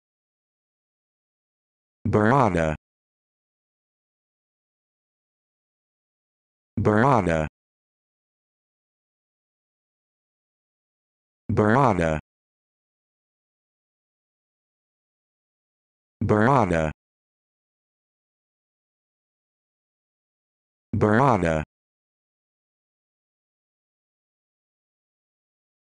How to Pronounce Burrata
This video shows you how to pronounce Burrata